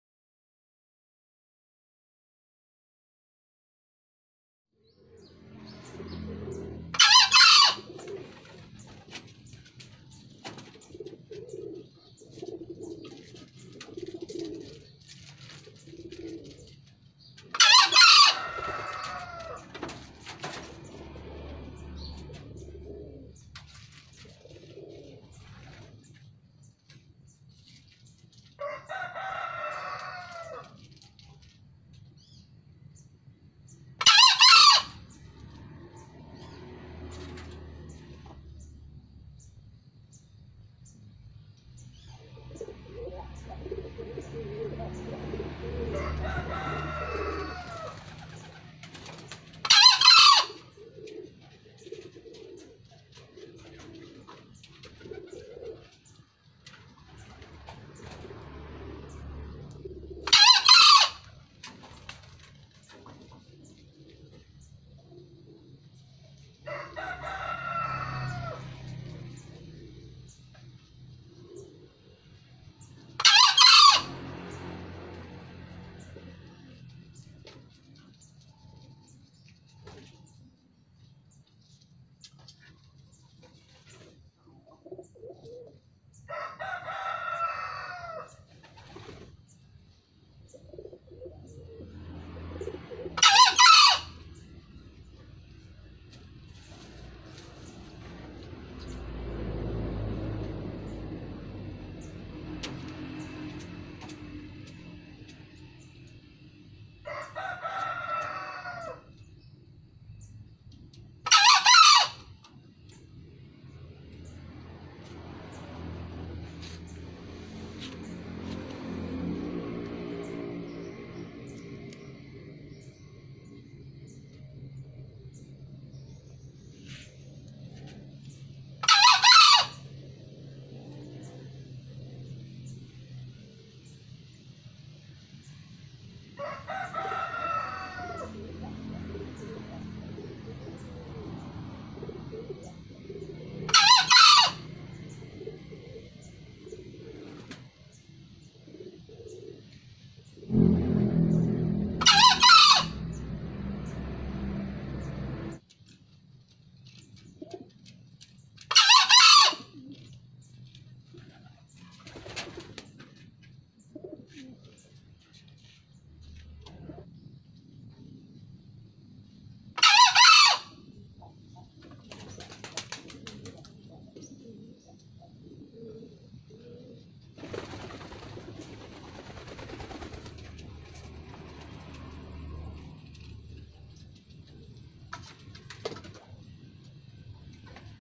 Ayam Hutan Hijau Berkokok Dari Kebumen
Ayam hutan Hijau biasanya di temukan di pulaunjawa dan Lombok.. Dan ini adalah ayam hutan asli dari Kebumen Jateng